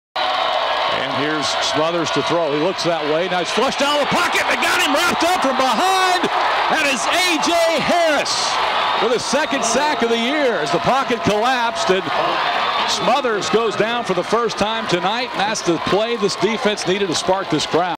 #42 Aaron Harris, LB, Southeastern Louisiana, Sack vs UCA, 10.27.12
Southeastern Louisiana LB, Aaron Harris, sacks Central Arkansas QB, Wynrick Smothers, 10.27.12